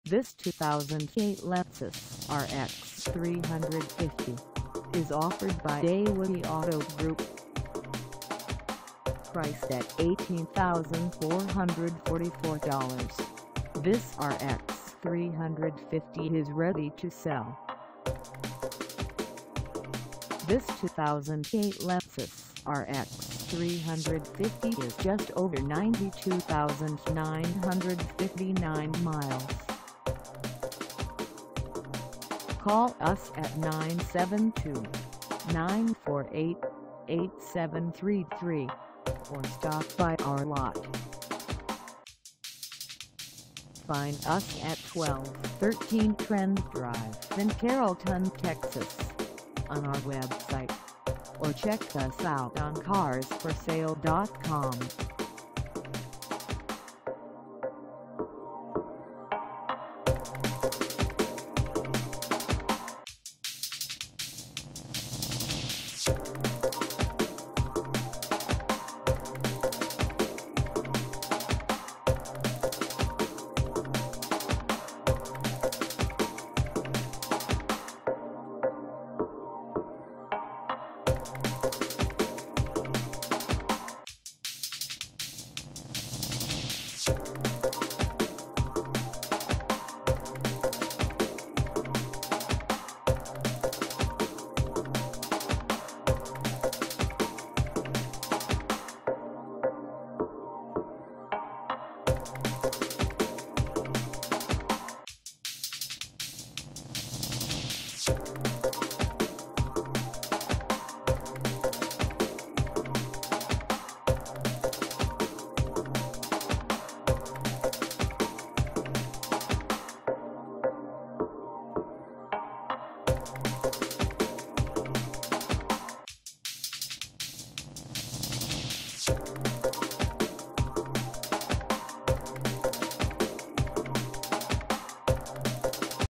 2008 Lexus RX 350 SUV for sale in Carrollton, TX 75006 at th
This 2008 Lexus RX 350 SUV is for sale in Carrollton, TX 75006 at eWay Auto Group.
Engine: 6-Cylinder
Transmission: Automatic
Color: Millennium Silver Metallic
Mileage: 92959
Contact eWay Auto Group 1213 Trend Drive Carrollton, TX 75006 to test drive this 2008 Lexus RX 350 SUV today.